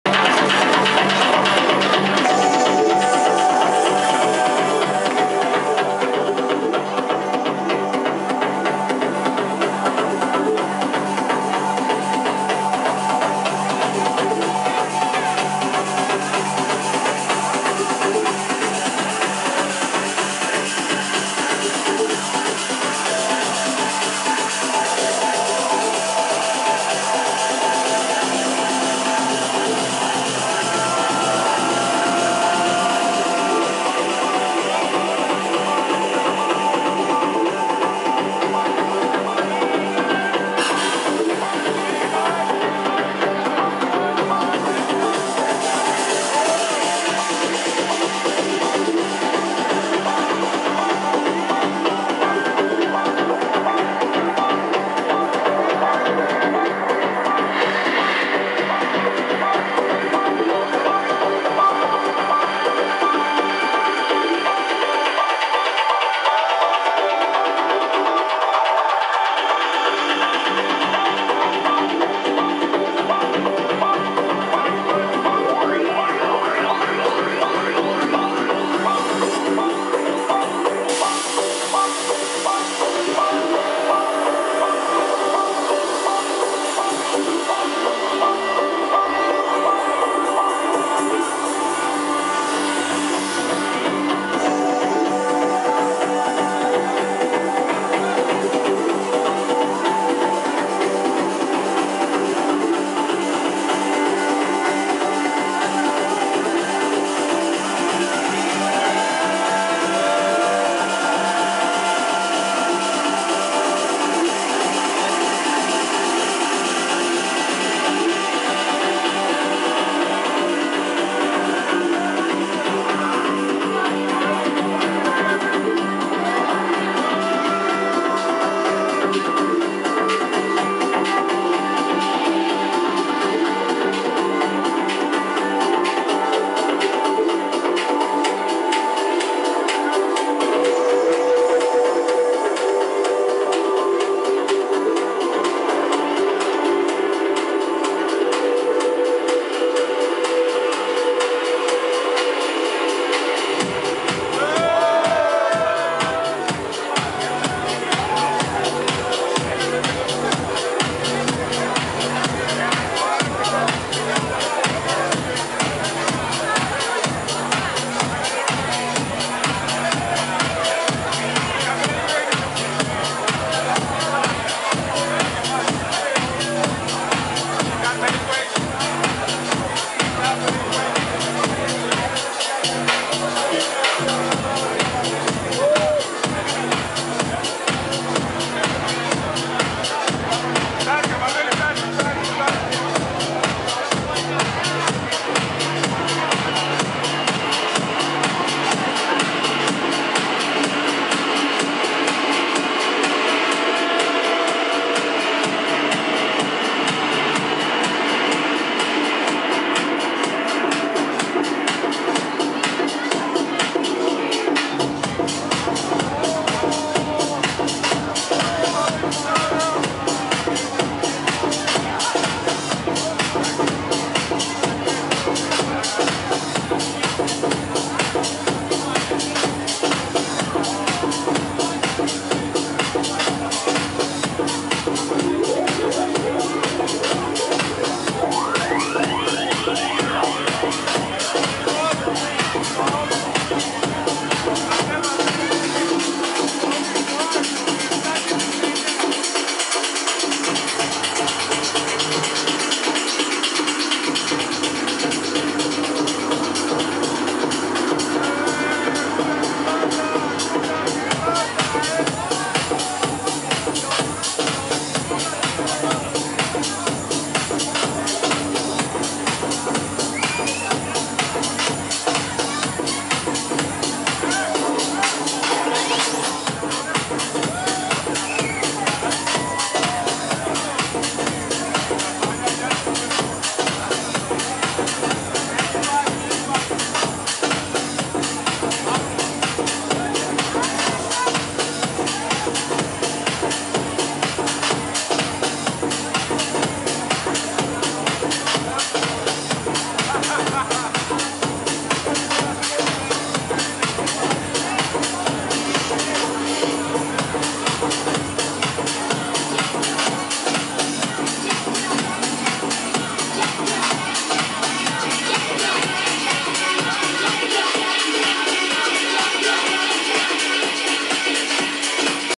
Robert Babicz pt1 @Mamacas, Athens
Robert Babicz pt1 @Mamacas
Robert Babicz gig w/Adrianos Papadeas & dPen @ Mamacas, Athens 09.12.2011